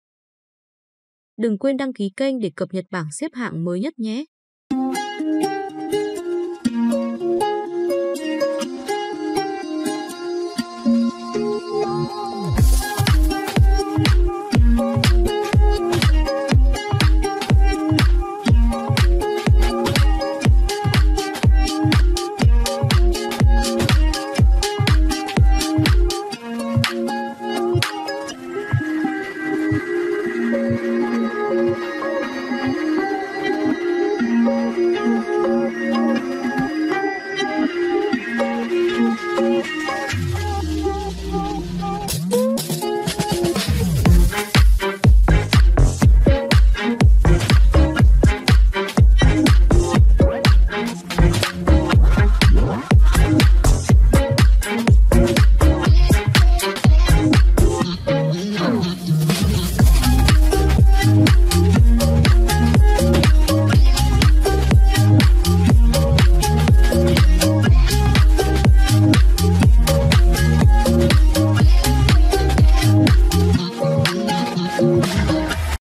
Bảng xếp hạng Đấu Trường Danh Vọng Mùa Đông 24/9/2021 mới nhất [ Liên Quân Mobile]
Bảng xếp hạng Liên quân Đấu Trường Danh Vọng Mùa Xuân 2021: Cập nhật kết quả, bảng xếp hạng Đấu Trường Danh Vọng Mùa Đông 2021 mới nhất.